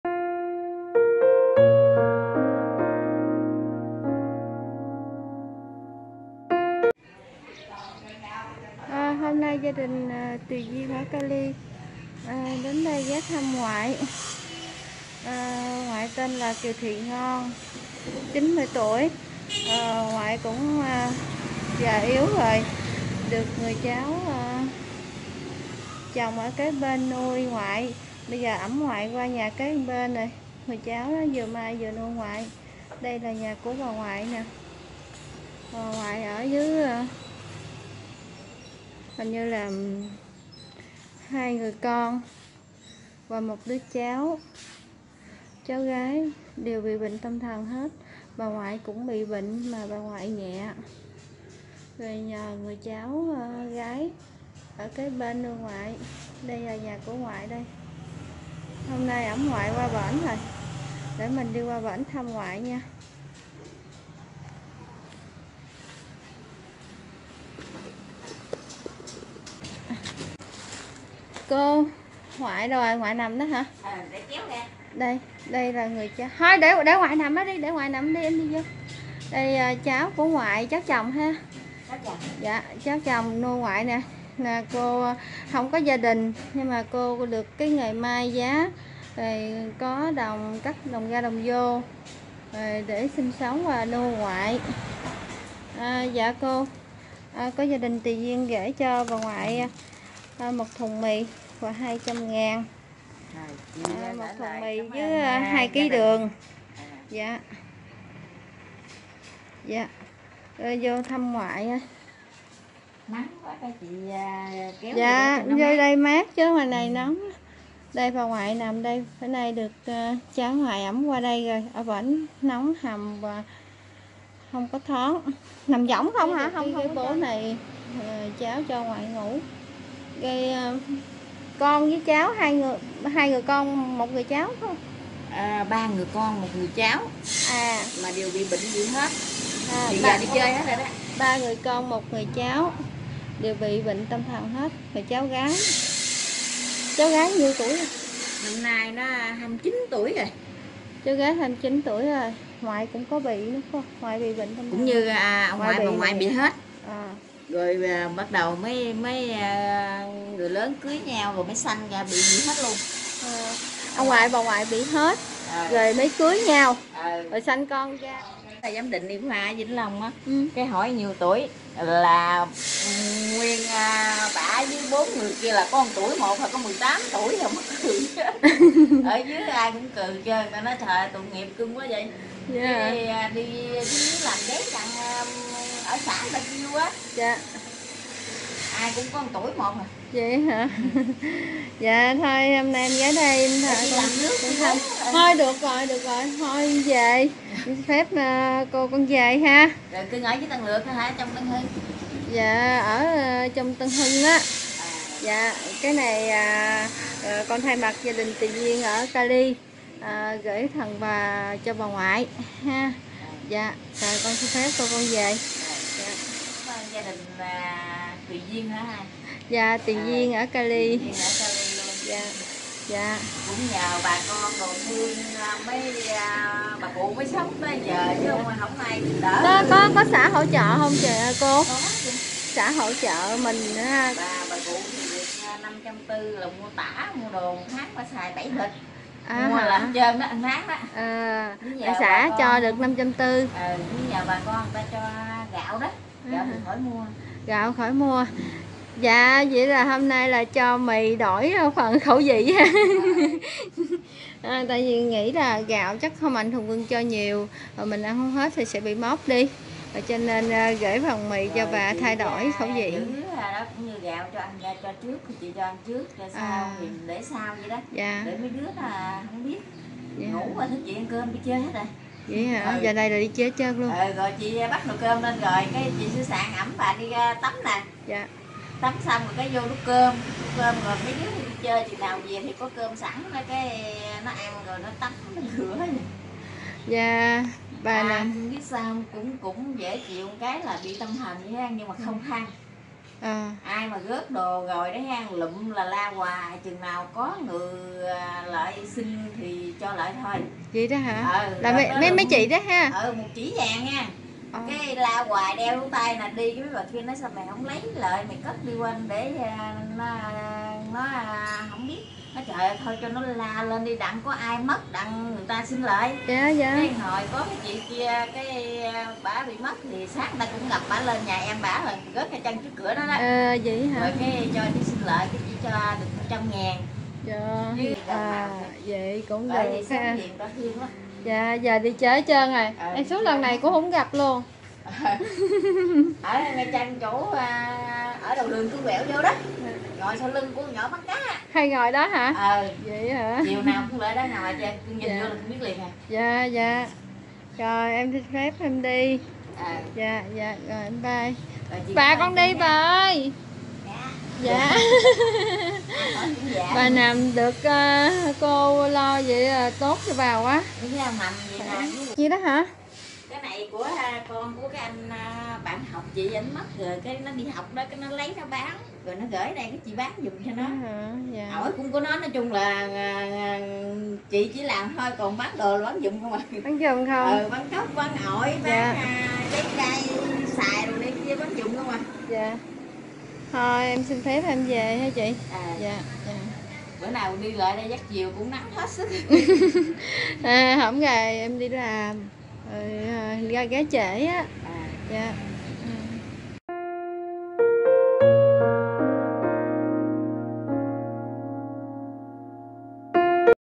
Bà Kiều Thị Ngon 90 Tuổi | Gia Đình Đều Bệnh Tâm Thần@TuyDuyenCALI
Hôm nay chia sẻ thêm một hoàn cảnh thật tội nghiệp, Bà cụ 90 tuổi, và con cháu đều bệnh tâm thần.
 Được cô cháu chăm sóc bà cụ kể lại là ngày xưa  ông bà cụ lúc trẻ cũng bị bệnh, kết hôn với nhau và sanh ra những người con cũng bị bệnh giống Ông Bà.
  Mỗi cây mỗi hoa, mỗi nhà mỗi cảnh, thương cho những hoàn cảnh bất hạnh.
    Cầu chúc bà cụ và gia đình bà được tai qua nạn khỏi bệnh tật tiêu trừ.